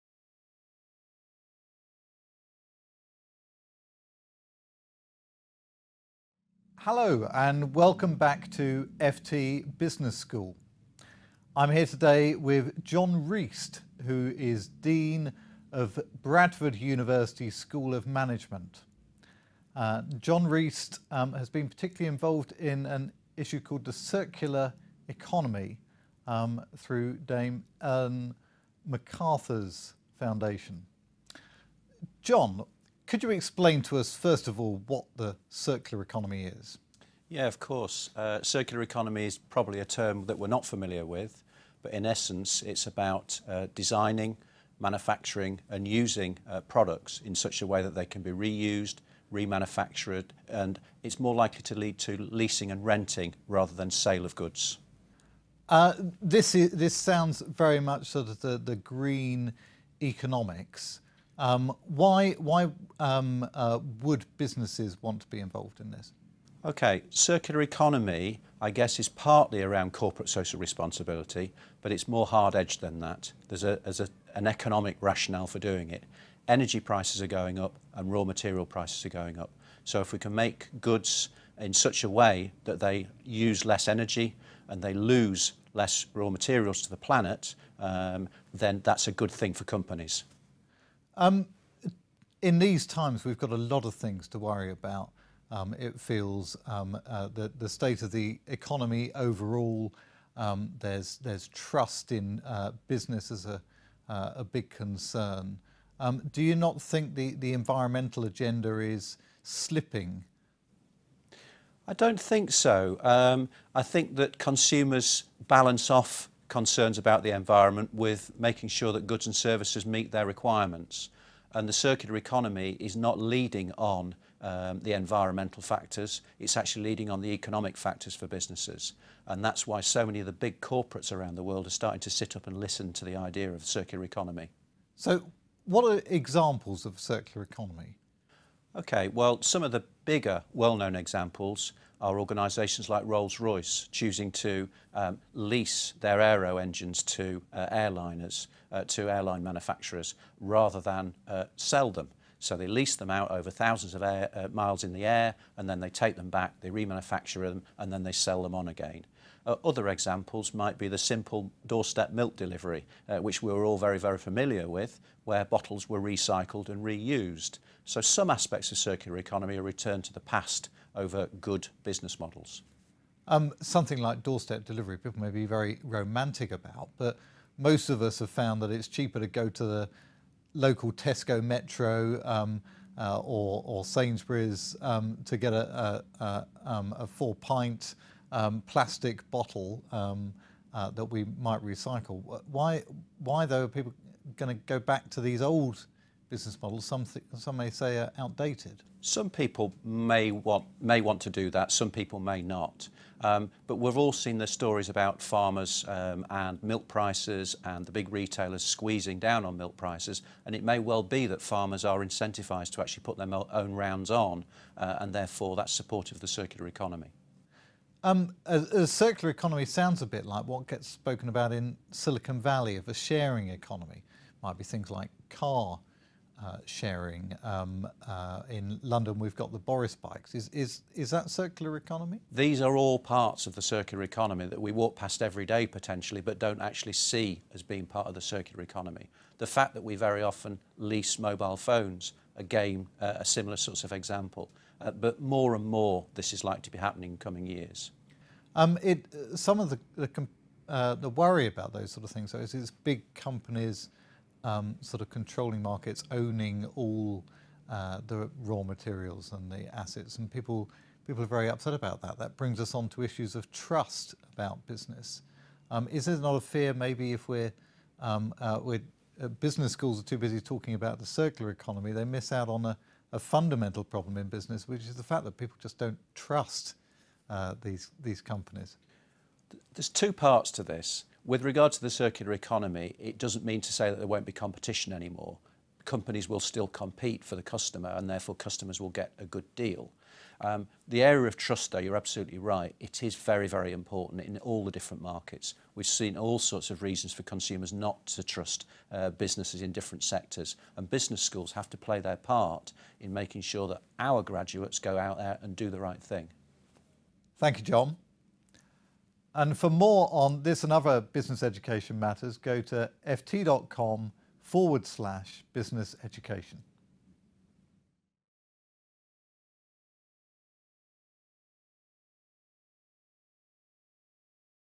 The hard edge of green economics
The re-use of products is being driven by economic rather than environmental factors, says Jon Reast. The dean of Bradford University School of Management tells Jonathan Moules that more companies now understand the benefits of the circular economy.